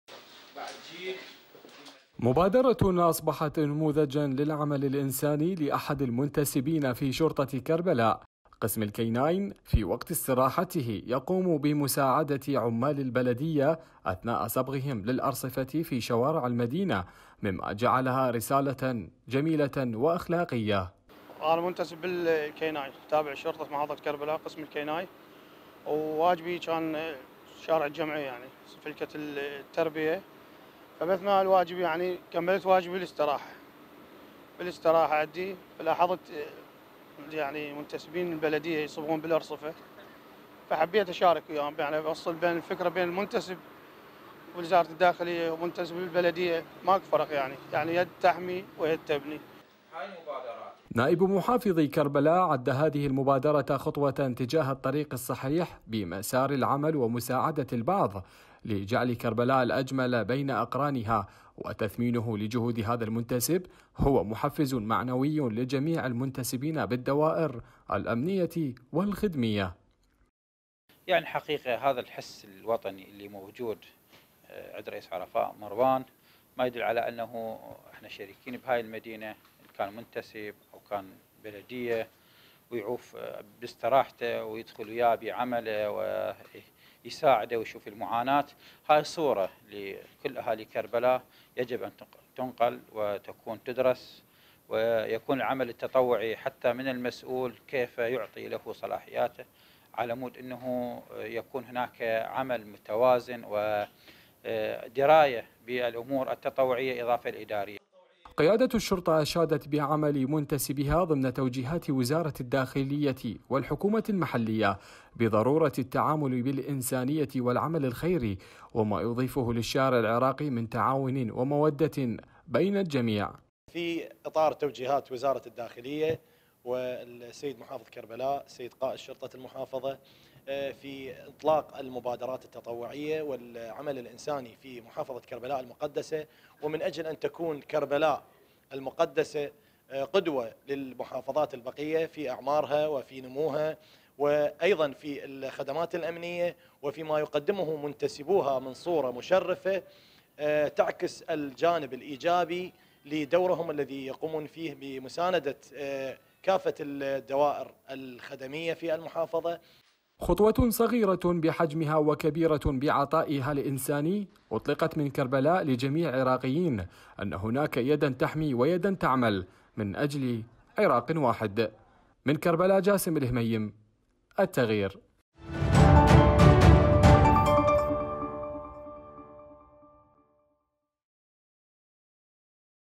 شرطي في كربلاء يضيف لمهامه صبغ الأرصفة | تقرير
منتسب في الشرطة يساعد عمال بلدية كربلاء في صبغ الأرصفة بعد الانتهاء من عمله مما جعله قدوة في المحافظة واصبح حديثه على مواقع التواصل الاجتماعي, مراسلان جاسم الهميم رافقه اثناء تكريمه
شرطي في كربلاء يضيف لمهامه صبغ الأرصفة
موقع قناة التغيير الالكتروني